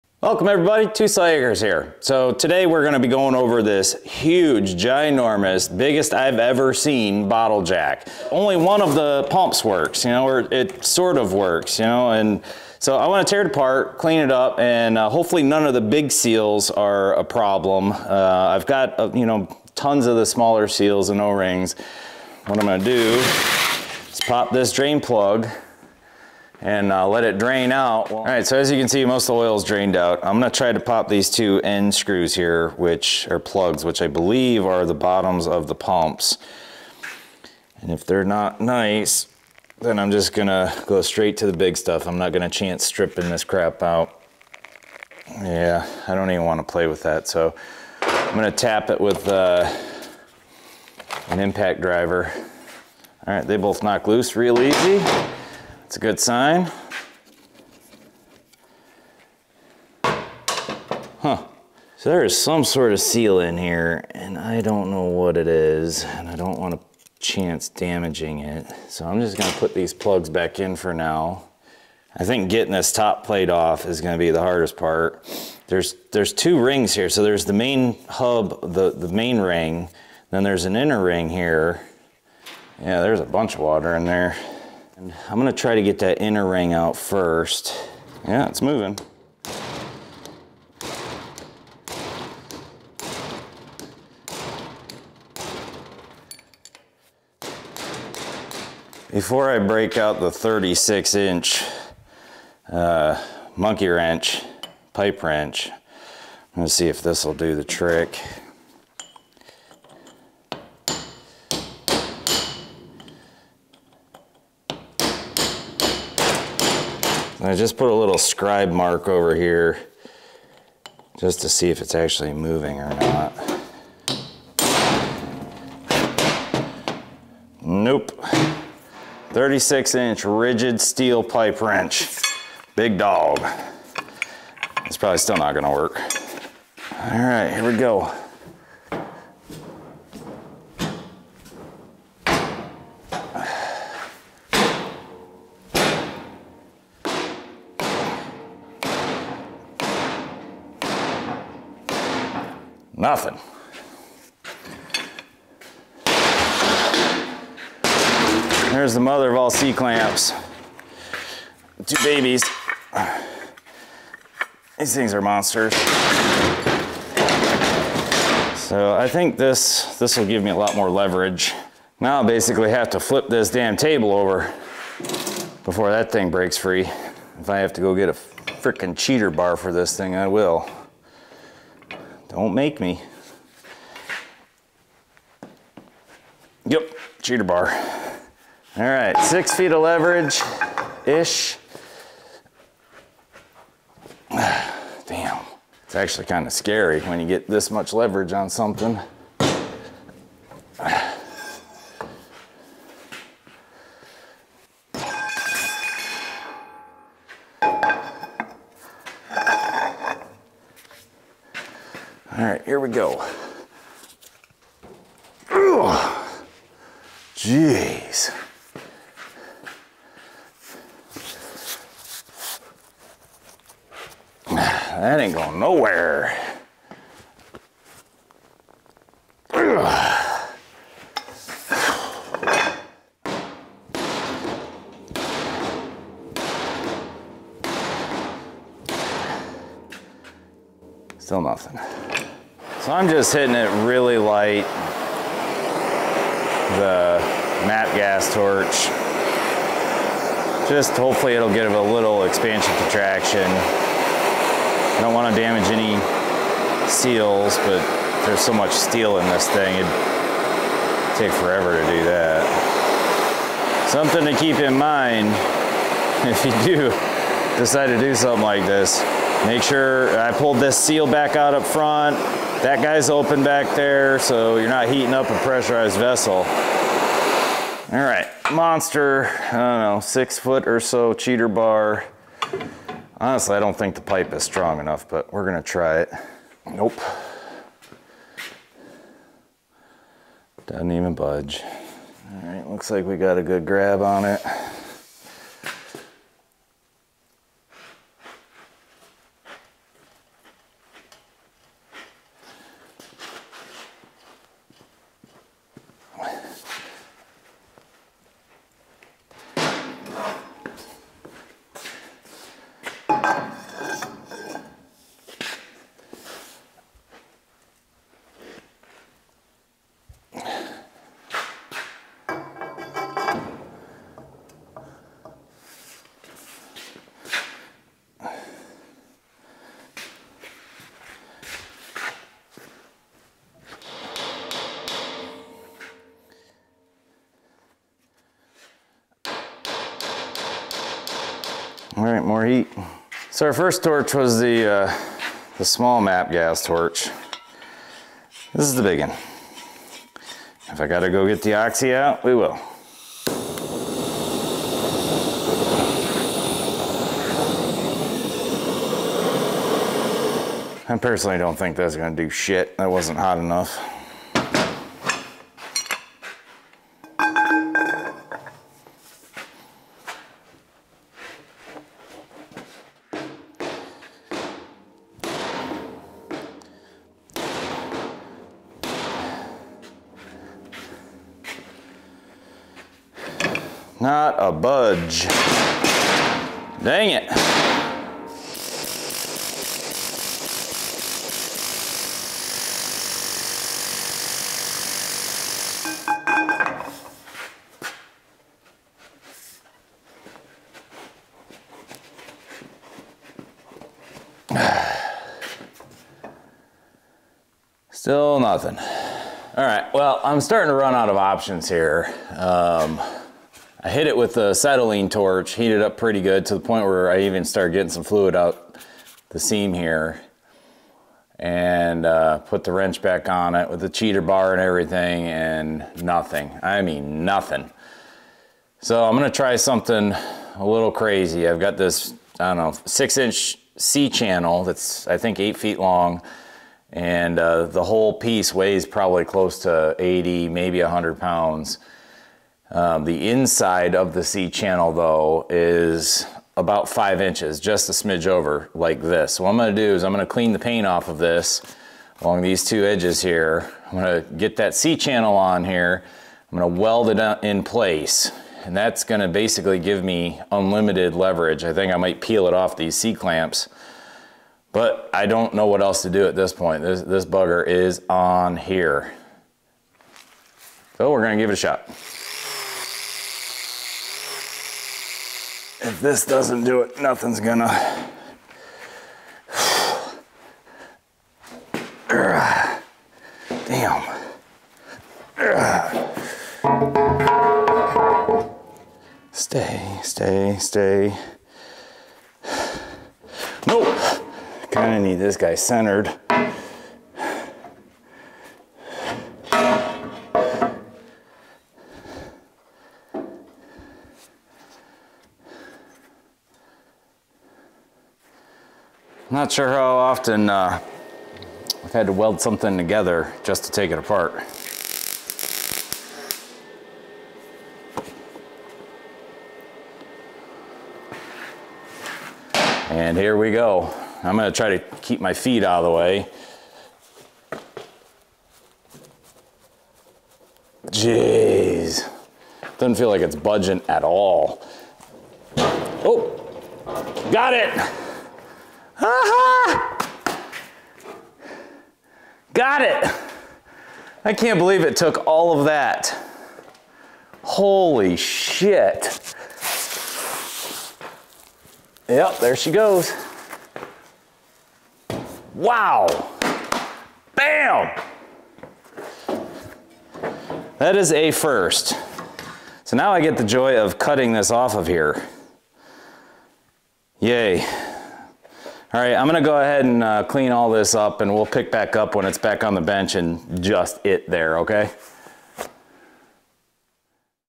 50 Ton Jack restoration part one
Full tear down, recondition and rebuild of a 100+ lb bottle jack, part one.  Breaking the top nut loose was the most difficult I've ever encountered.